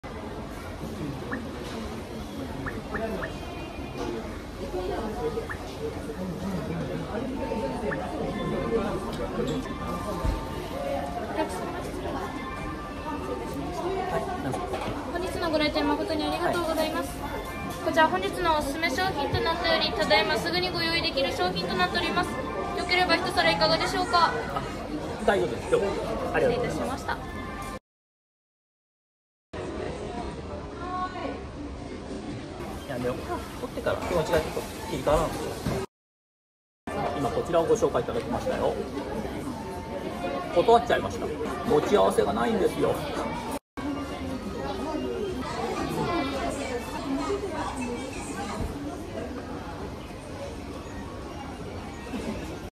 相棒 杉下右京 ものまね撮影中ハプニング
官房長との回転寿司ネタでの裏側。
お店のご迷惑にならないよう神経をすり減らしました笑